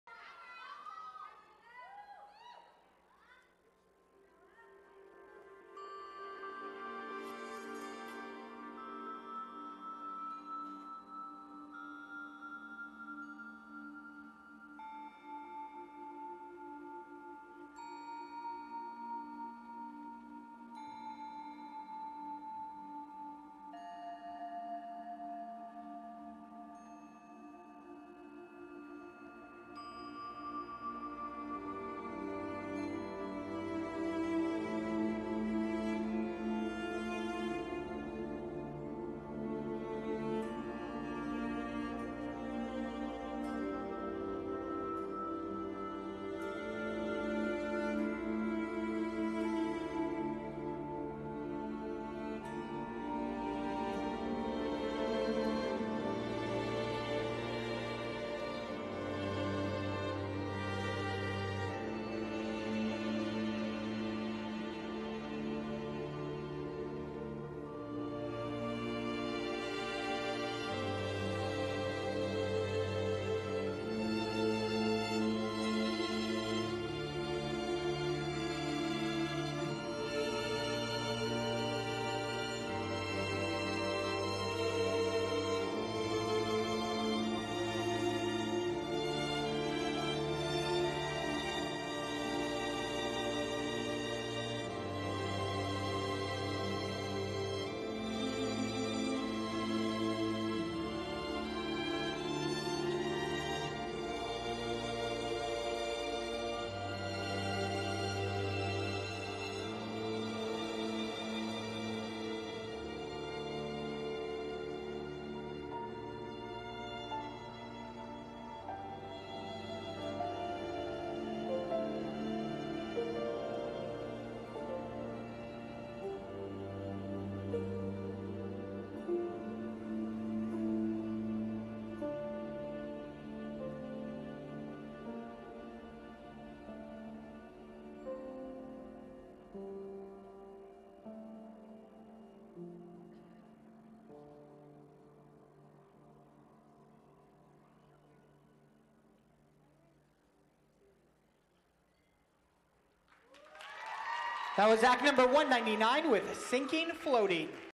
Rhythm Dance Center- Sinking, Floating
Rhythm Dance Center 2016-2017
Junior Contemporary Group
Choreography: Sarah Brinson
Dancers: Eva Ardagna, Ryleigh Carder, Kennedy Dickson, Srah fuchs,  Alexandra Jinglov, Kathryn Lester, Connor Mason, Bella Mills, Kimmy Torres,
24Seven Atlanta